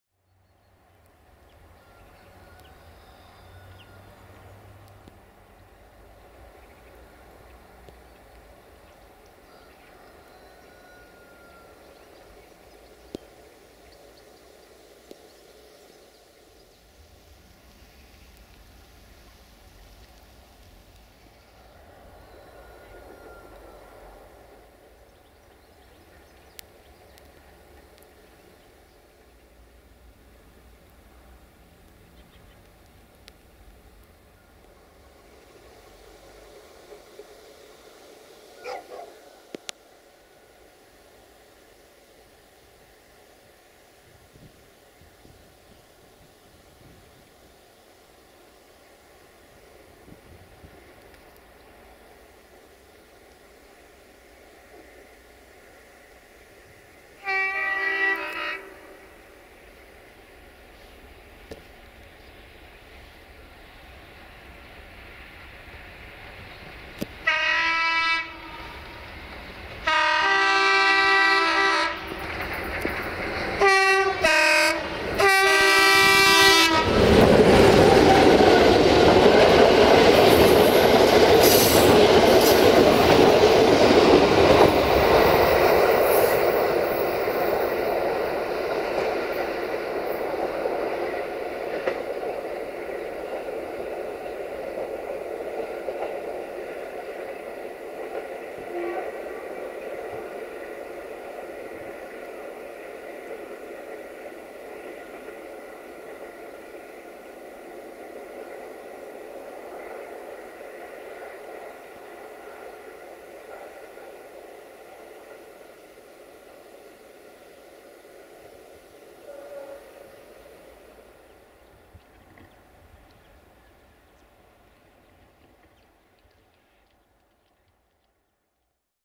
060-DA 60-1115-4 & IR1834 Timișoara Nord-Iași in Oradea Est Triaj - 16 January 2020
060-DA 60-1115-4 & IR1834 Timișoara Nord-Iași in Oradea Est Triaj - 16 January 2020

1: IR1834 - Timișoara Nord - Arad - Chișineu Criș - Salonta - Oradea - Huedin - Cluj Napoca - Dej Călători - Beclean pe Someș - Ilva Mică - Vatra Dornei - Suceava - Pașcani - Iași - 060-DA/LDE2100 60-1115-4 RO-SNTFC

22 January 2020